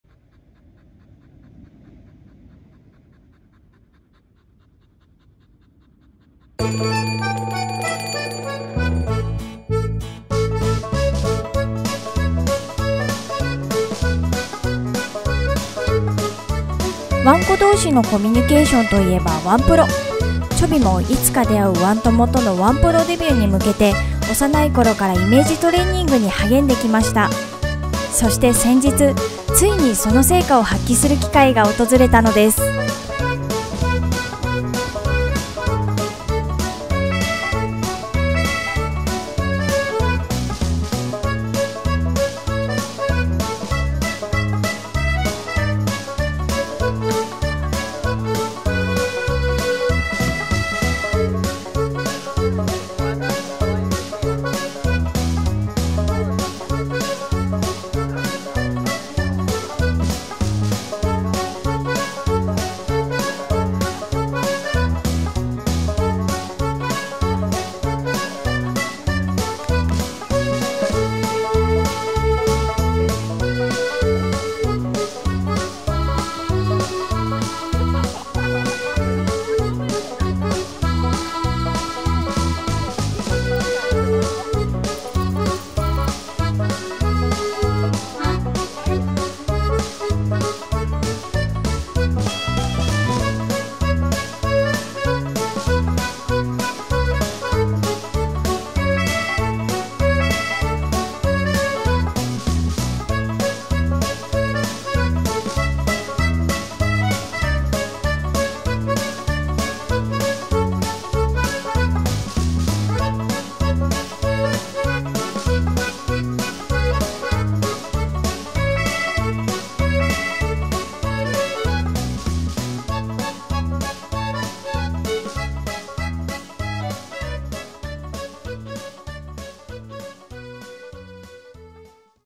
【#13】初ワンプロで現実の厳しさを思い知らされるゴールデンレトリバー子犬
チョビの憧れ、ワンプロデビューの日は突然やってきました。
以前、先輩わんこの激しいワンプロを目撃してから、クッション相手のイメトレが激化していたチョビ。
クッション相手では余裕でねじ伏せていたチョビですが、
実戦ではこれ如何に？